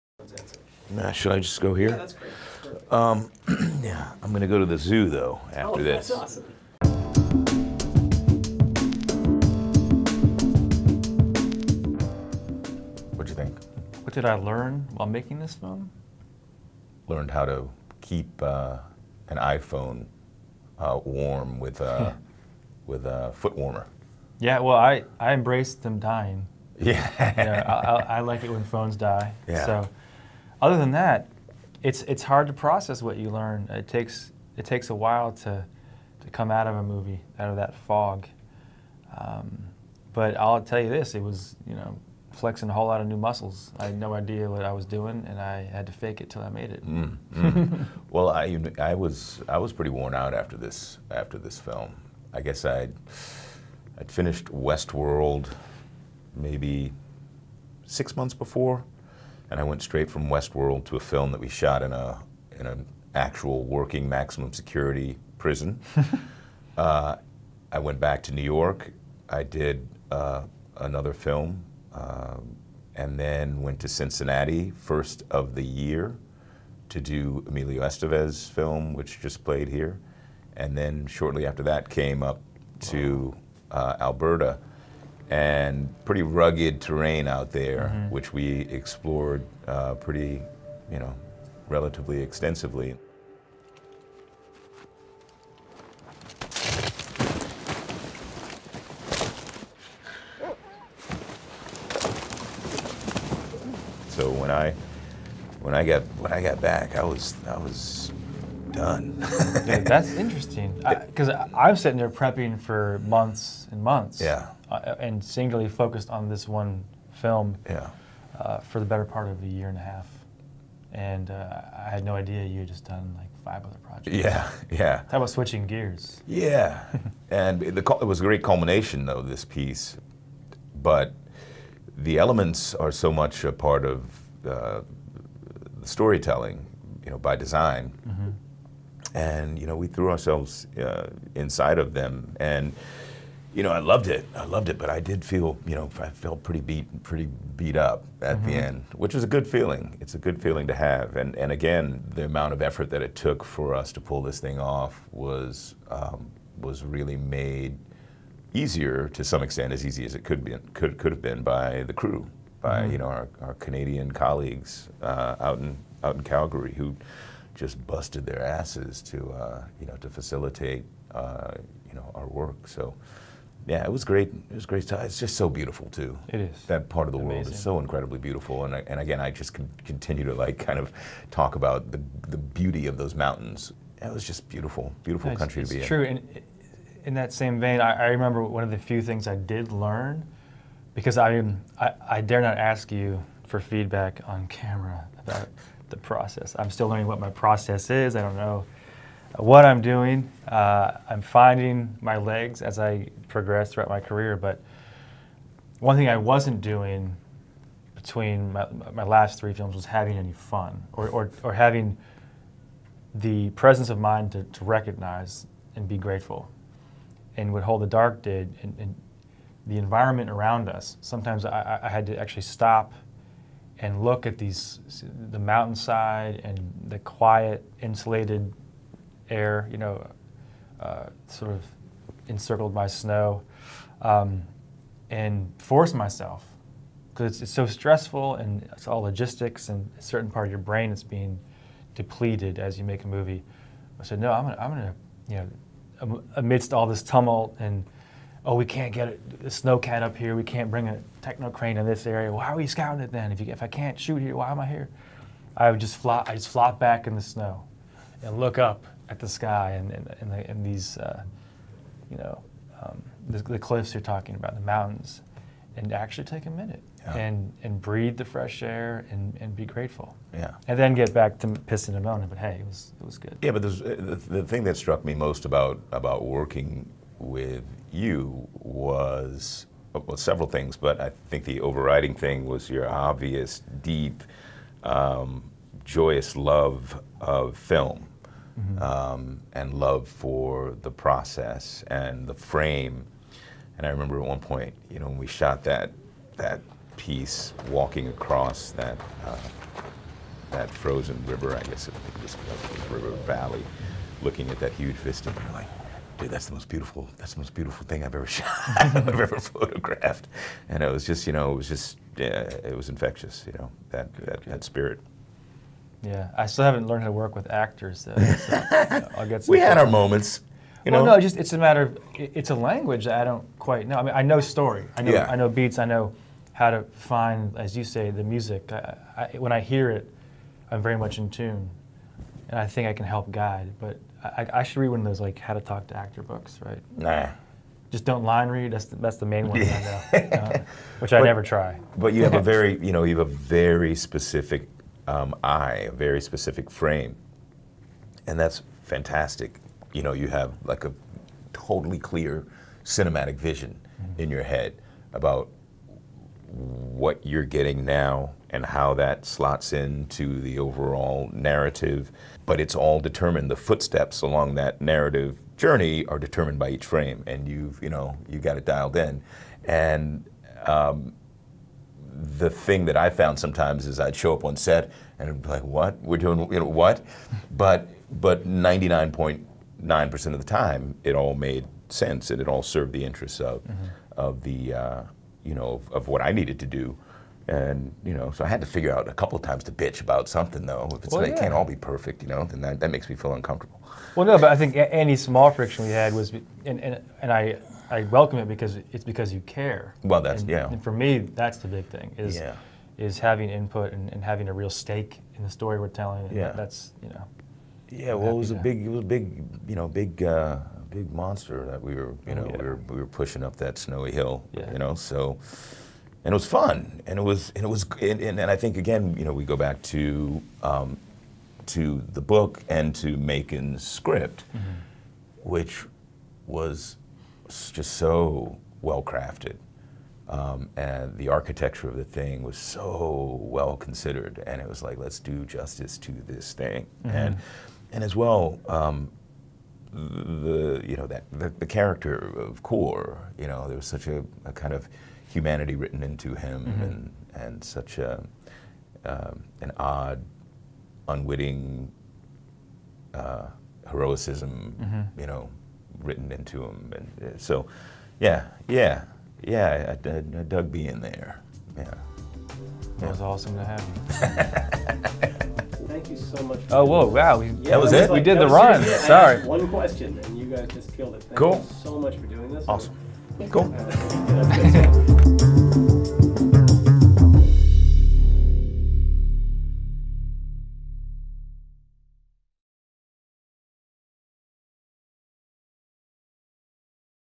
How HOLD THE DARK's Jeffrey Wright and Jeremy Saulnier Survived a Canadian Winter | Talent x Talent
Jeffrey Wright (WESTWORLD) and Jeremy Saulnier (GREEN ROOM) sit down in TIFF's Studio 9 to discuss their latest film HOLD THE DARK, the beauty of the Canadian rockies, and how best to keep an iPhone working in the freezing cold.

A child is missing. His home is one of a handful of trailers on the edge of the wilderness. His father (Alexander Skarsgård) is serving in the Middle East and his mother (Riley Keough) seems to be succumbing to cabin fever. She calls in Russell Core (Jeffrey Wright), a writer and expert on wolves; she believes the creatures took her boy and hopes Core can find him. Core accepts the mission as a pretext to visit his estranged daughter in Anchorage, but quickly realizes he's taken on a stranger and more sinister task that he could have anticipated. He finds himself in the middle of both a long-simmering dispute between a disenfranchised Indigenous community and the local authorities, and a mass-murder investigation. Adapted from William Giraldi's novel by actor and scenarist Macon Blair — who has appeared in each of Saulnier's features — Hold the Dark is equal parts neo-noir and ancient myth.